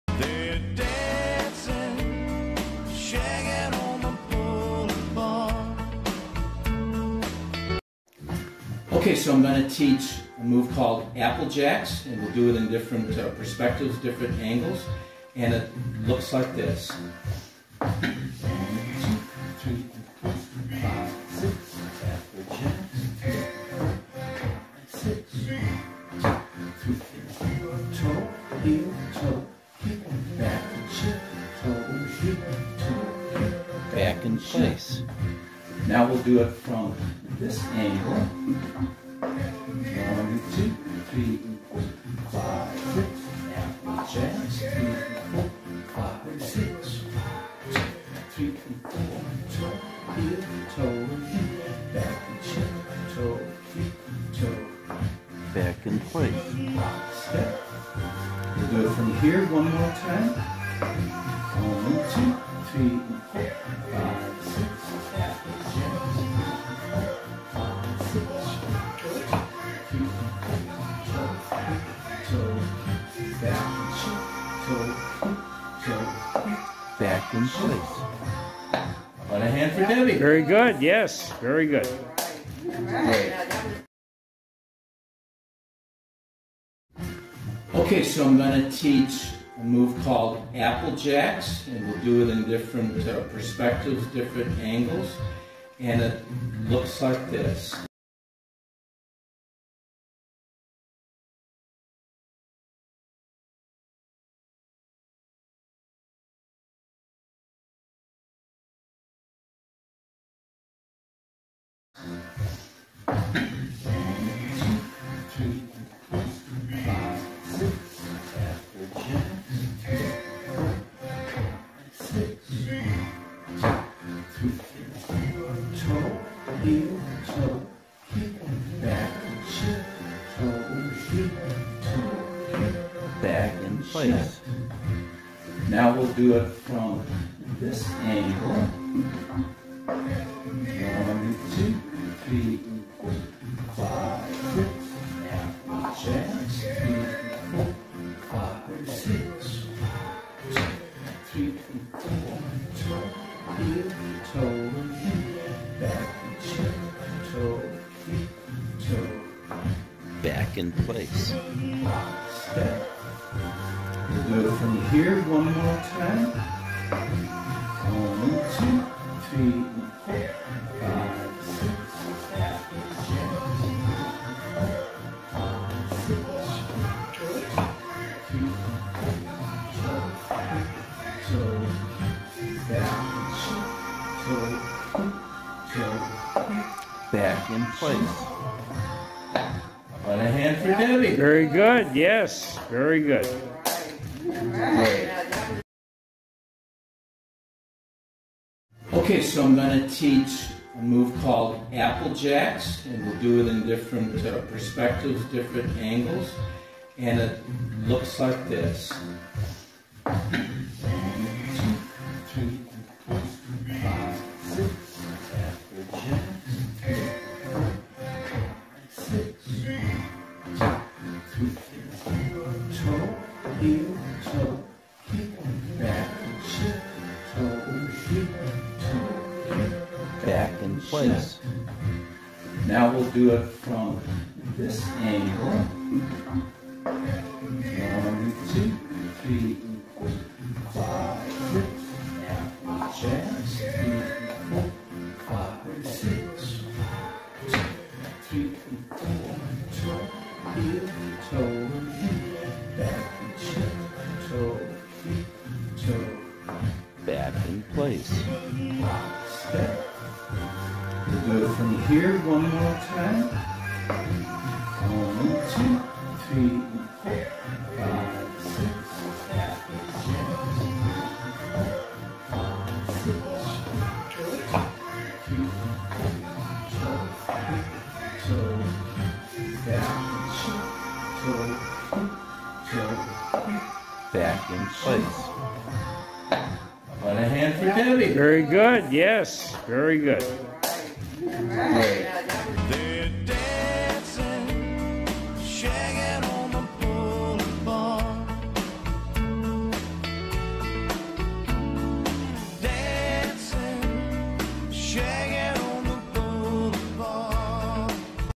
Carolina Shag Instructional Video #33 "Applejacks." Jimmy Flynn with Ms. Debbie Biondo.
Carolina Shag Instructional Video #33.  "Applejacks" Together Step.
Jimmy Flynn, with Ms. Debbie Biondo

Cleveland Area Shag Club (CASC).